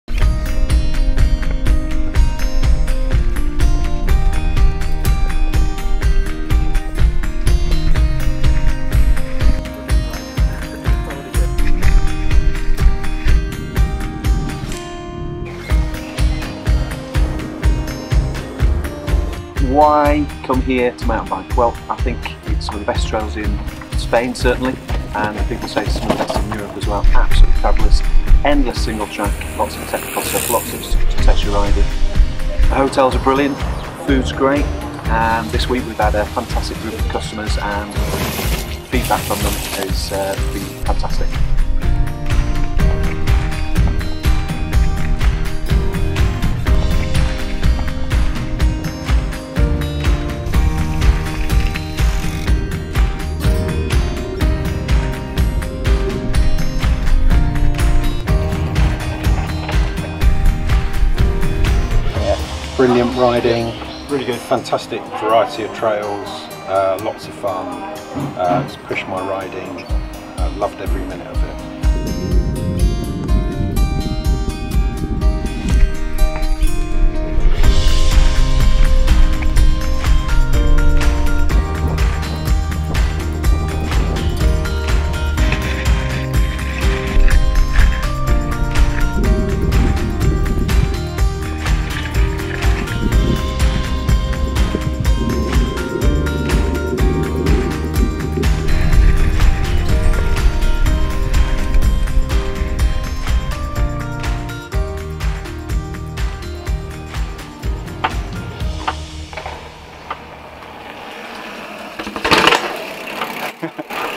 Have you heard about Spain's epic Sierra Nevada trails?
Join us on a thrilling and technically demanding journey through Spain's mighty Sierra Nevada Mountain range.